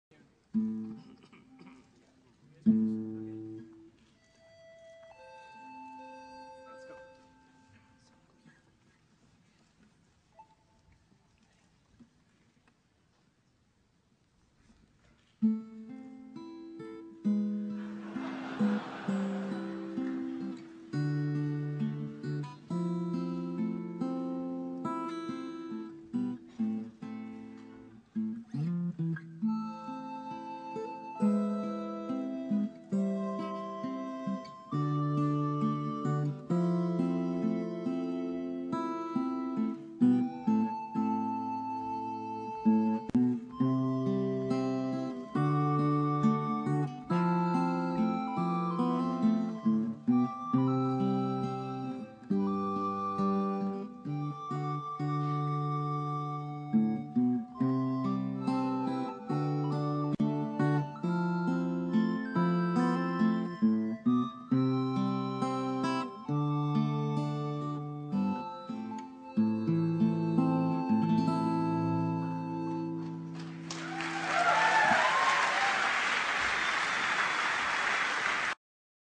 Standford mobile phone orchestra
Standford mobile phone orchestra performance in Crunch Award 2009.